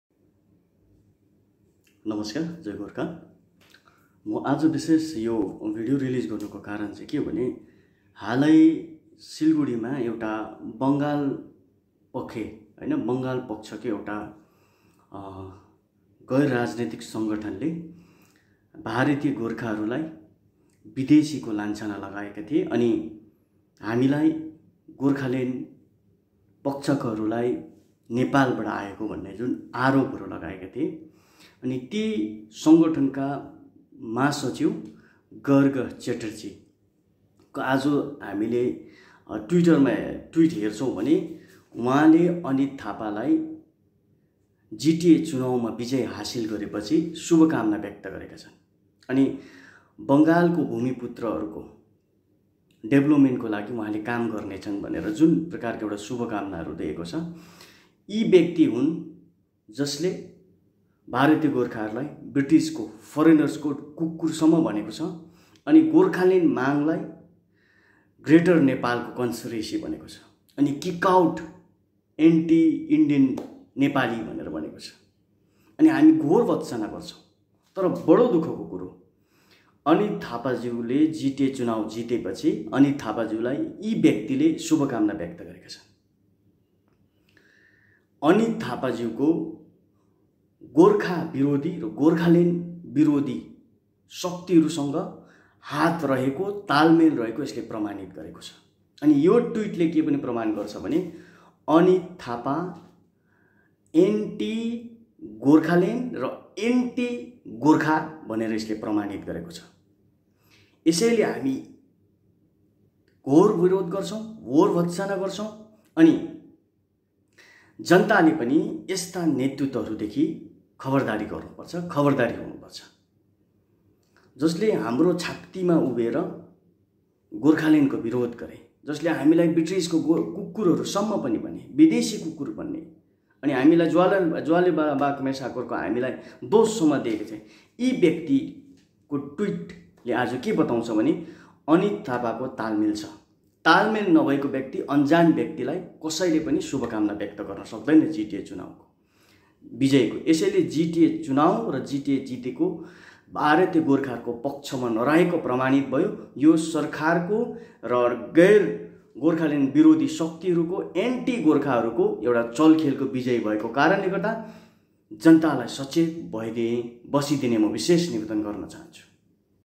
GTA Election is victory for anti Gorkha Elements.
Gagar Chatterjee, general secretary of Bengal Pkkho of the anti-Gurkha group in Siliguri, has congratulated Anit Thapa on his victory in the GTA elections. This means that there is evidence that Anit Thapa has links with anti-Gorkhaland elements. This is a matter of great sadness.